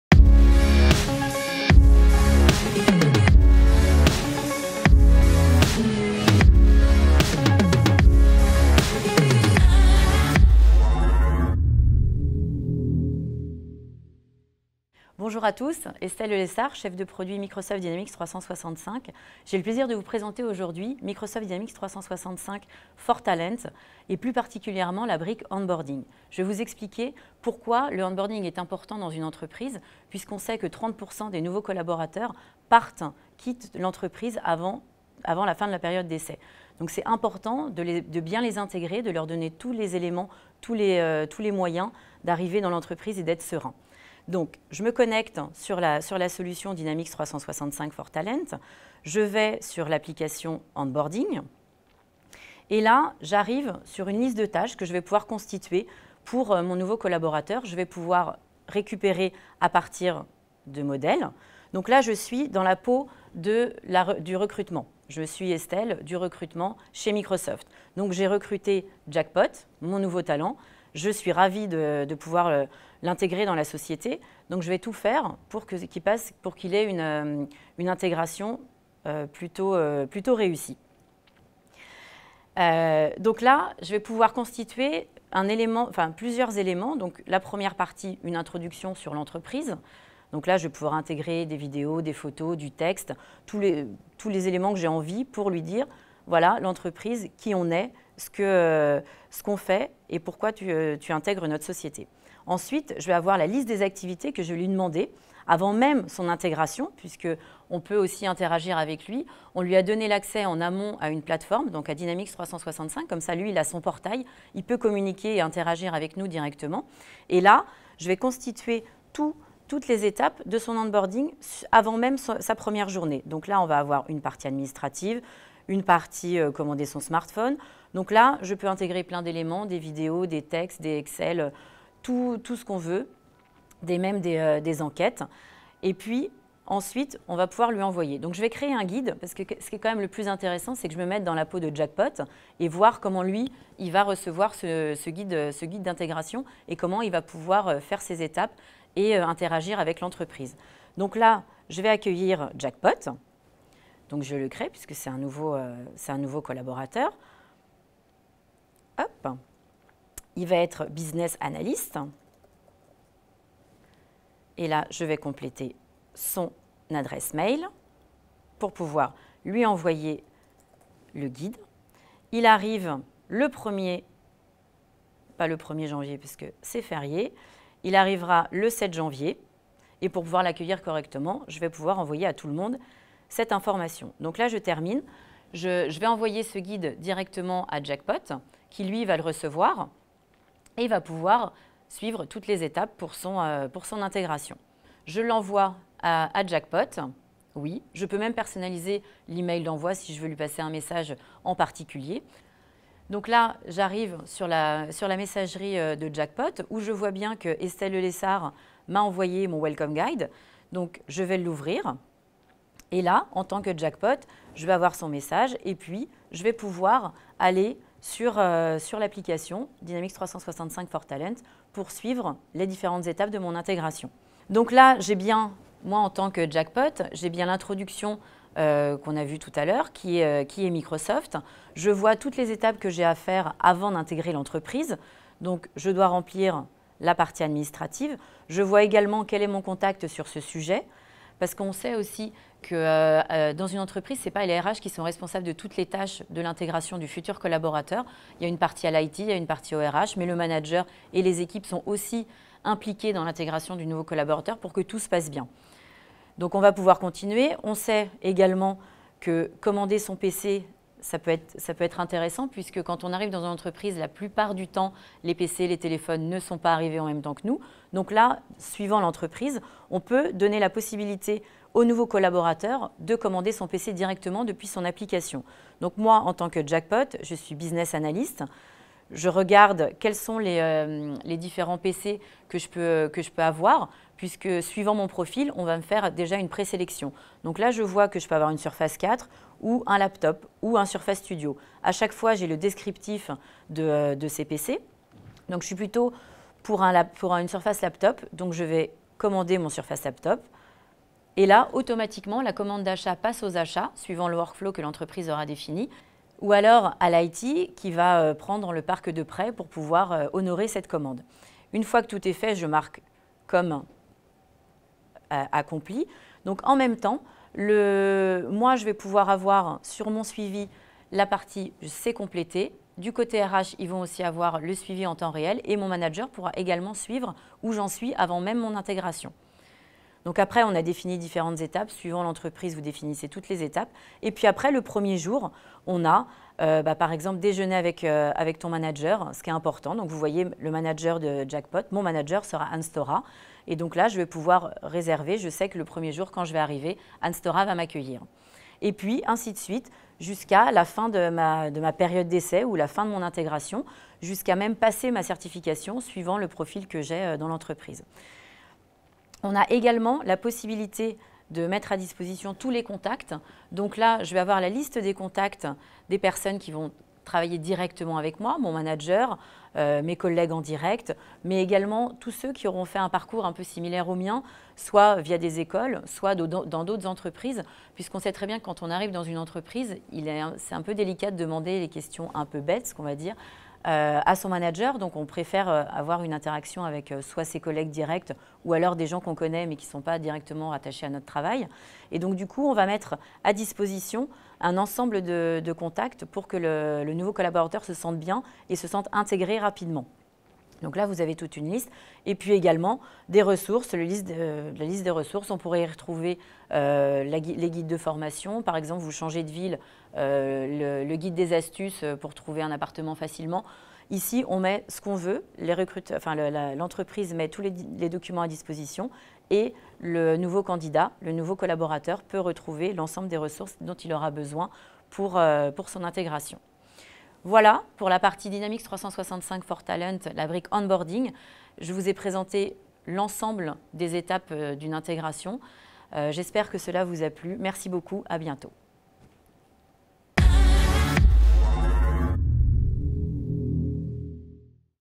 [DEMO] Onboarding des Talents avec Dynamics 365
Estelle Delessard, Chef de produit Dynamics 365, présente les fonctionnalités de la solution "Dynamics 365 for Talent" (dédiée aux populations RH) et plus particulièrement la brique "Onboarding" qui permet de mieux accueillir et former les nouveaux talents intégrant votre entreprise.